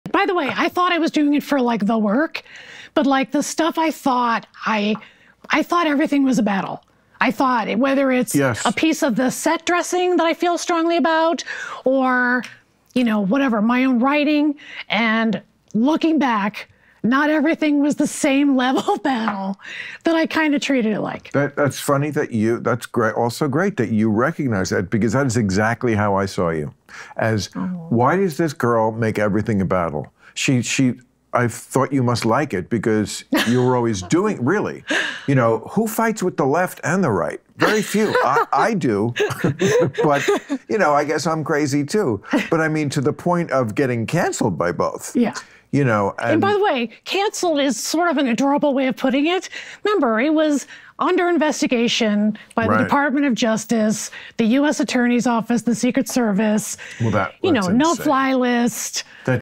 Kathy Griffin on getting Cancelled by both sides
Bill Maher and Kathy Griffin on why Kathy makes everything a battle, Kathy’s fight with MAGA nation, what it’s like to be cancelled, how the Trump photo happened, the brilliance of Joe Rogan, the upsetting Kennedy scene in the Marilyn Monroe movie, all the people who sued Kathy, and Bill’s pitch for a TV show for Kathy.